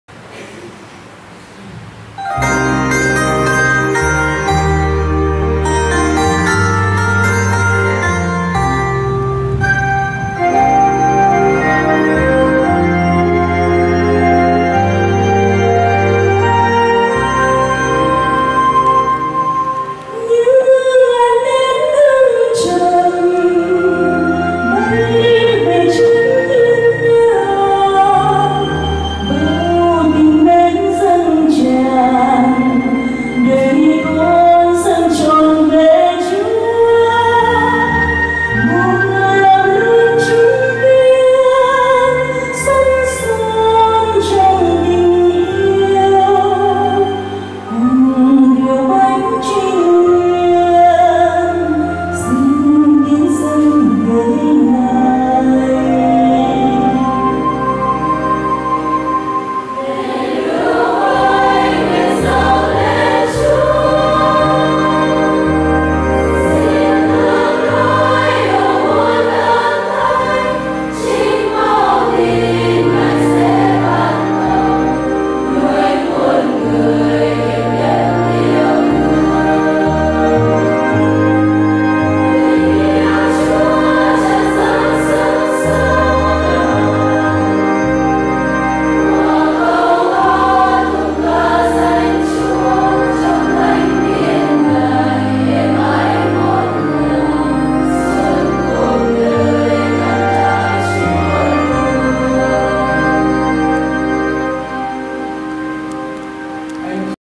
Lễ Các Em Vào Tiền Tập & Vào Tập Viện. Dâng Lễ
Thánh Lễ Được Cử Hành Lúc 5h Sáng  Tại Hội Dòng Con Đức Mẹ Mân Côi Trung Linh  Ngày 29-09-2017. Do Cha Linh Hướng Dòng Chủ Tế. Cha Nguyên Bề Trên Dòng Thánh Tâm Huế Cùng Đồng Tế